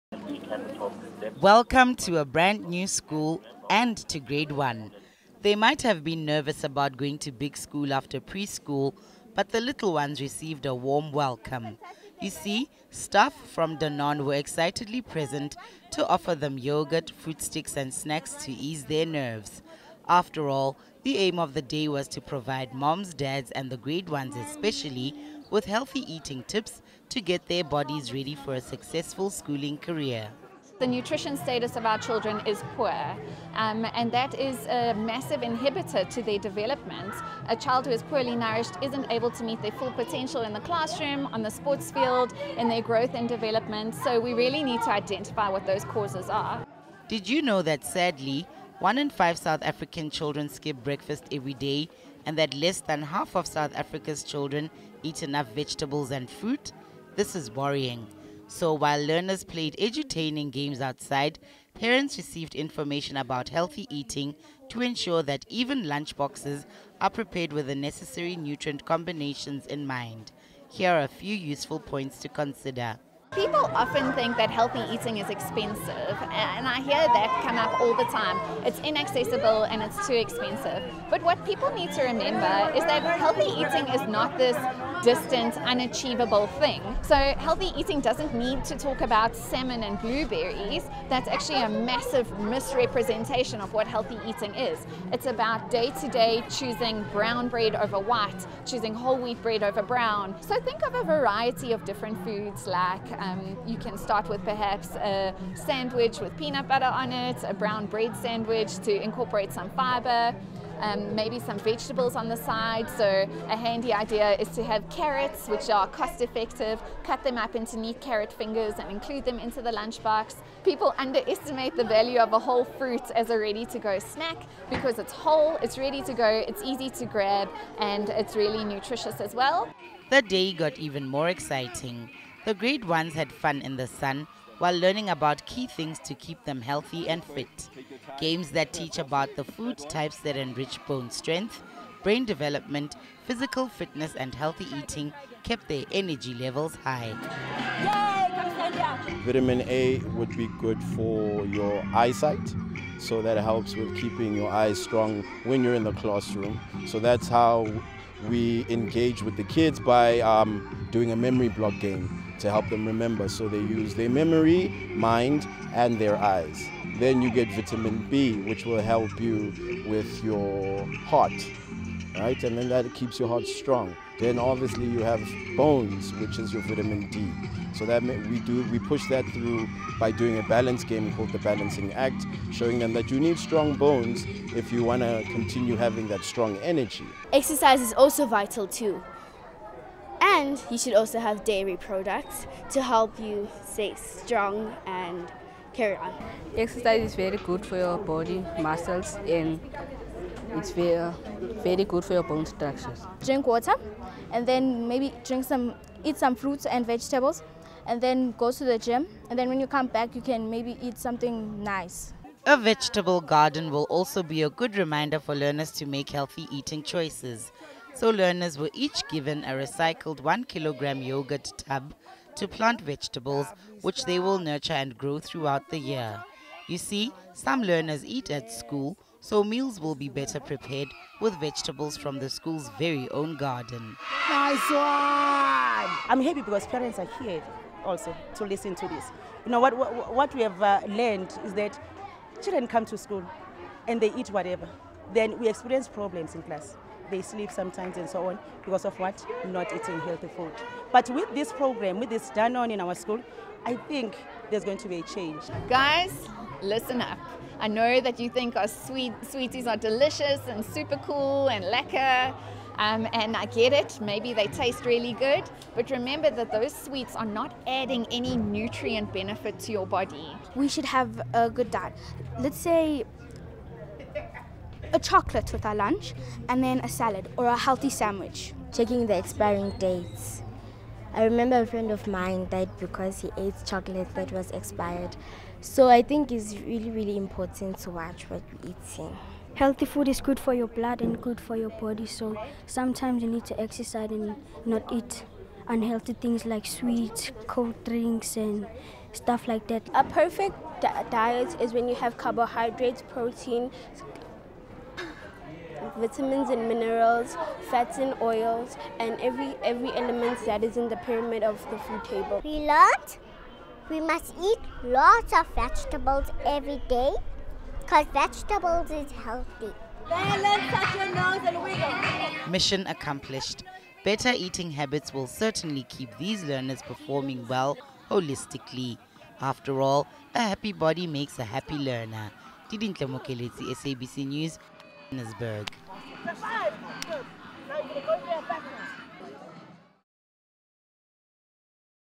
Kids News | Healthy eating plays an important role in ensuring that you do well at school
Did you know that healthy eating plays an important role in ensuring that you do well even at school? Well, the school year began with very important nutritional lessons for learners at Actonville Primary in the East of Johannesburg. Danone's 2020 Day One campaign empowered both parents and children, in a fun day full of activities.

Kids News’ Didi Mookeletsi joined them for a different day of learning.